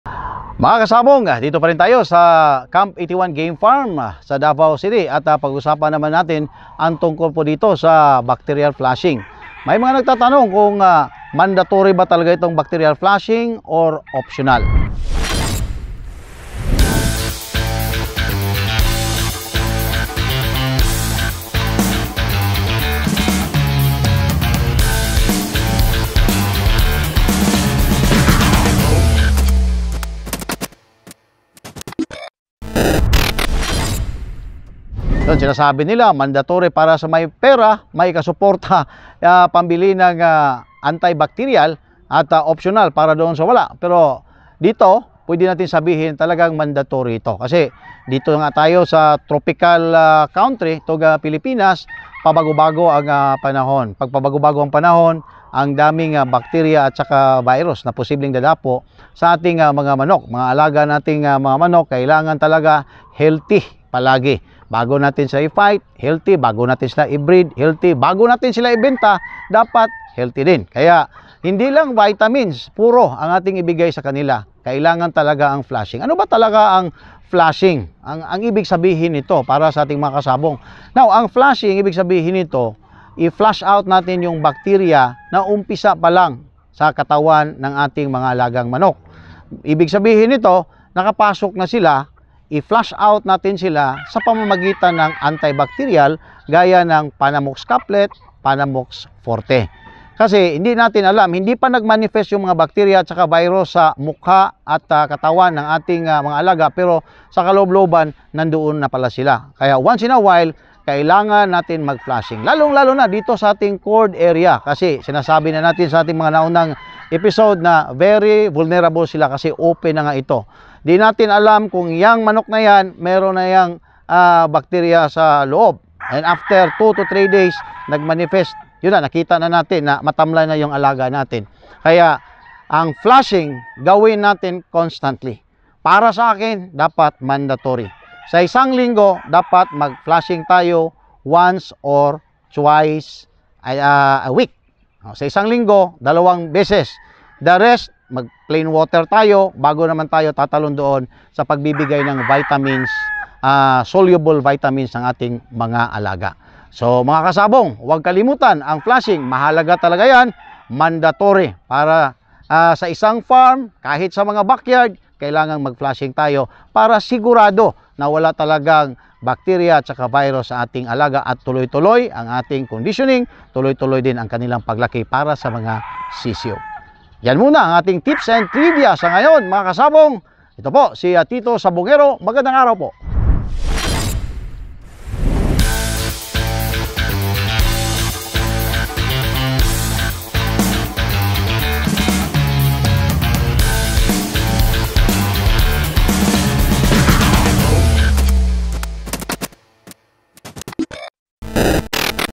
BACTERIAL FLUSHING MANDATORY O OPTIONAL | T2 SABUNGERO
Gaano ka ba kadalas na mag bacterial flushing? Alamin sa Tips and Trivia ni T2 Sab! [EP 10]